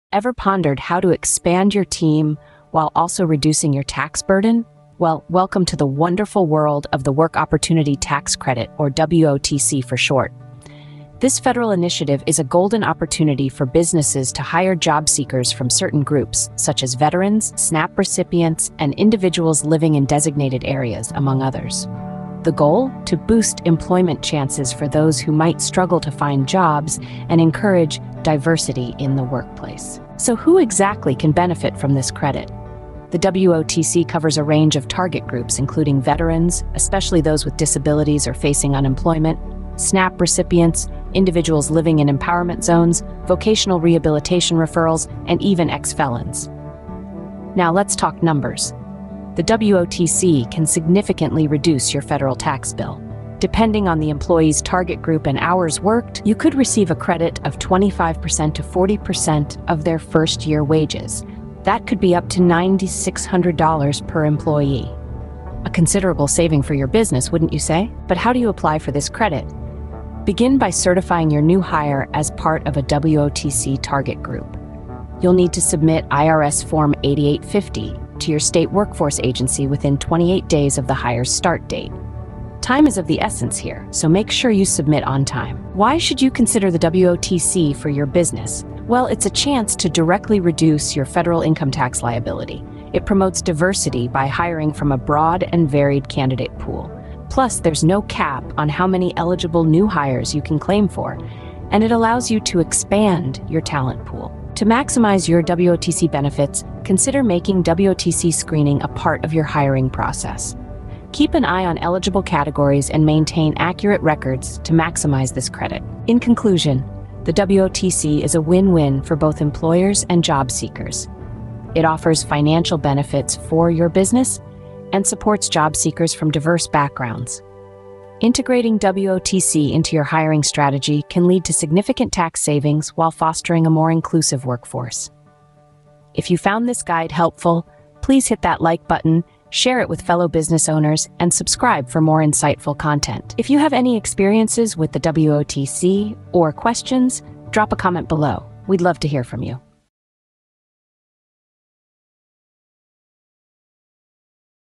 Understanding the Work Opportunity Tax Credit: A Guide for Employers
🚀 Unlock the Power of Hiring: The Work Opportunity Tax Credit Explained! 🚀

Welcome to our latest video where we dive into the Work Opportunity Tax Credit (WOTC) - a game-changer for businesses of all sizes! If you're an employer looking to expand your team and optimize your tax savings, this video is tailor-made for you. The WOTC is a federal tax credit that encourages companies to hire individuals from certain groups who face employment challenges. From veterans to SNAP recipients, this credit covers a wide range of potential hires, opening up a world of possibilities for both employers and job seekers.

In this video, you'll discover:

What the WOTC is and how it benefits both employers and employees.
Who qualifies for the WOTC, including detailed insights into the eligible target groups.
How much you can save with the WOTC, with examples of potential tax credits ranging up to $9,600 per eligible employee.
Step-by-step application process to ensure you successfully claim the WOTC.
Pro tips for maximizing your WOTC benefits, integrating it seamlessly into your hiring process for maximum savings and efficiency.
Why Watch?

Reduce Your Tax Liability: Learn how to significantly lower your federal taxes.
Enhance Workplace Diversity: Understand the importance of diversifying your workforce through inclusive hiring practices.
No Cap on Savings: Discover how there's no limit to the number of eligible hires, meaning unlimited potential for tax credits.
Expand Your Hiring Pool: Get insights on how the WOTC encourages considering a broader spectrum of job candidates.
Don't miss out on this opportunity to grow your business while contributing positively to the workforce. The WOTC isn't just a tax credit; it's a chance to make a real difference.

Have any questions or want to share your WOTC experience?